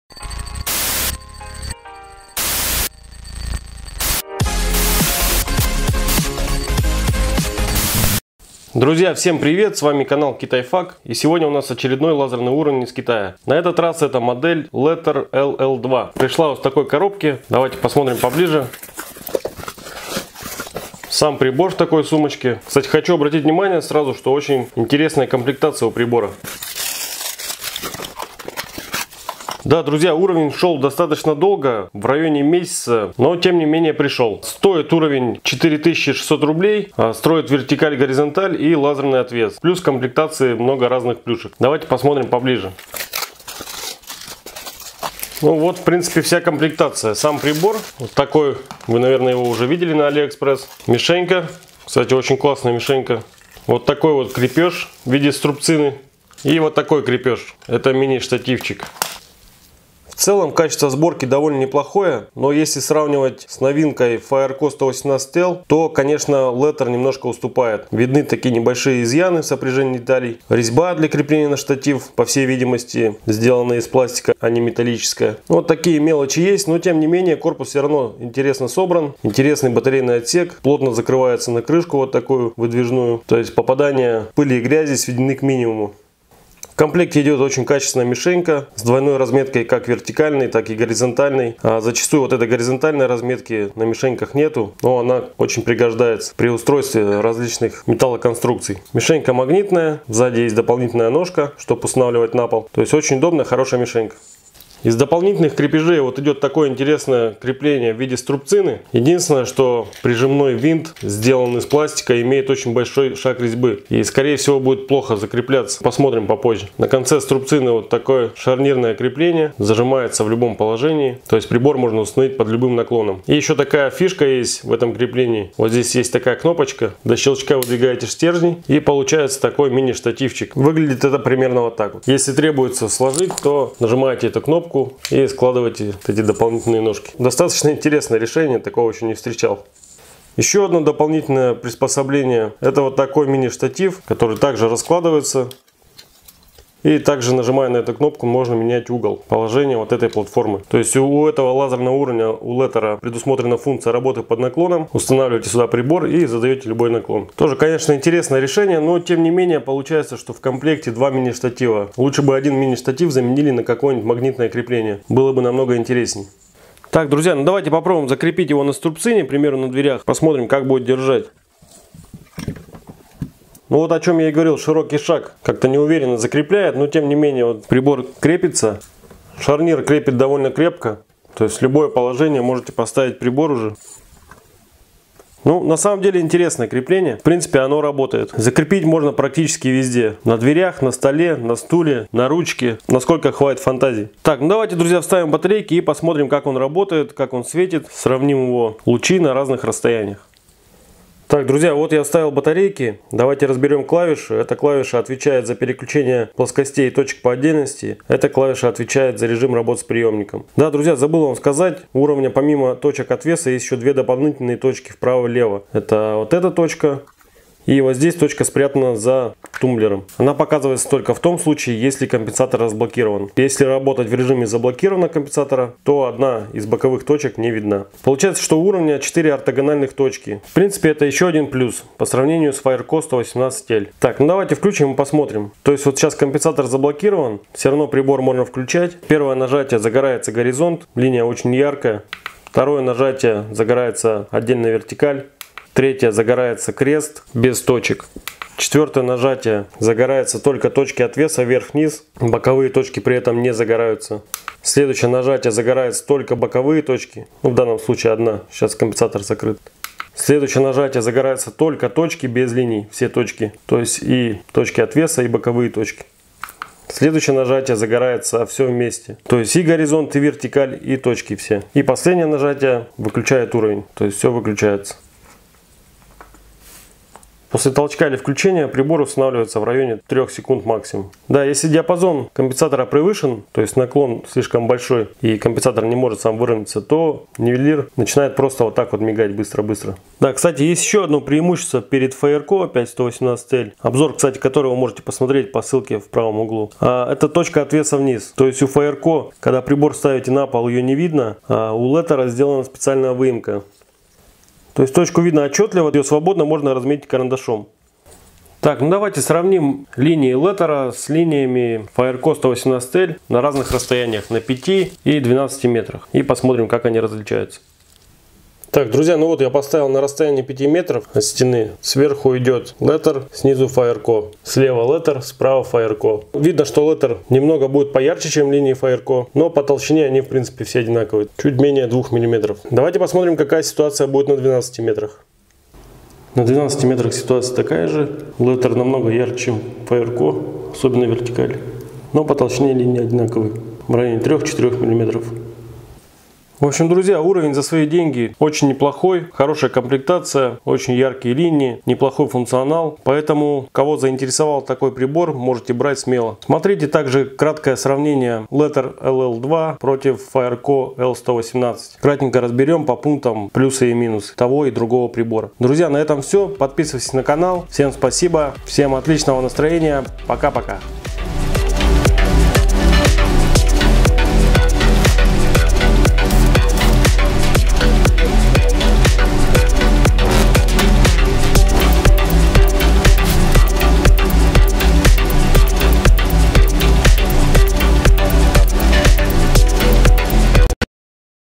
Обзор лазерного уровня из Китая - LETER LL2
Если нужна ссылка на официального продавца смотрите её в моём телеграм канале в этом посте
Интересный китайский лазерный уровень, строит две яркие плоскости (вертикальную и горизонтальную) и 4 взаимно перпендикулярных точки, в том числе две точки полноценного отвеса.

Лазерный нивелир LETER LL2 отличается не стандартной комплектацией, в которую входят не совсем обычные аксессуары для нивелира, а именно струбцина трансформер, которая превращается в маленький штатив и ещё один компактный складной мини-штатив с наклоняющийся площадкой.